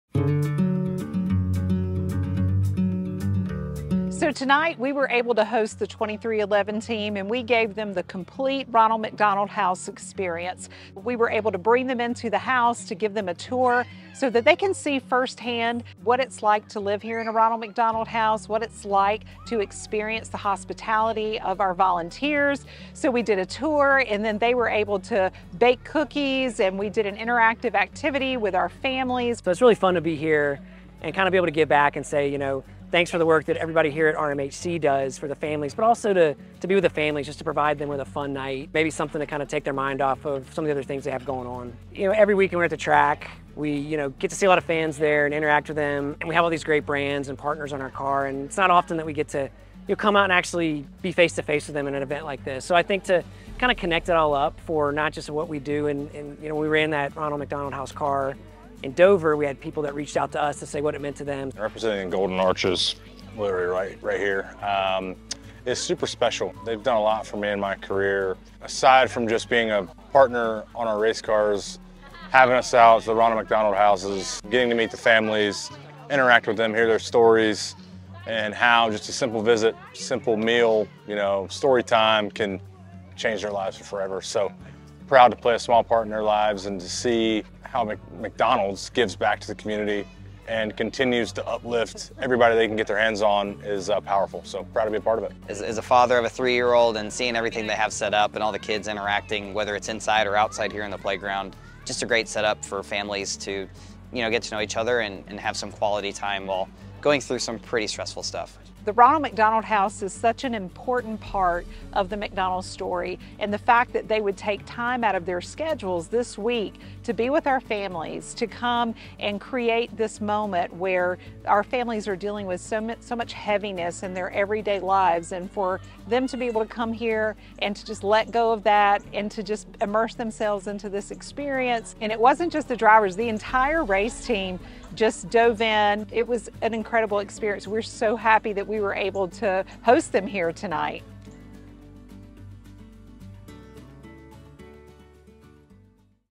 23XI Racing with Bubba Wallace and Tyler Reddick
RMHC of GC hosted 23XI Racing with NASCAR drivers, Bubba Wallace and Tyler Reddick, for a "Racing Into Summer" party with families! The drivers and their team served a delicious dinner, with cookie decorating, playground fun, and music from our Split Second Sound DJ Smylee. Good luck to Bubba and Tyler this weekend at the Charlotte Motor Speedway!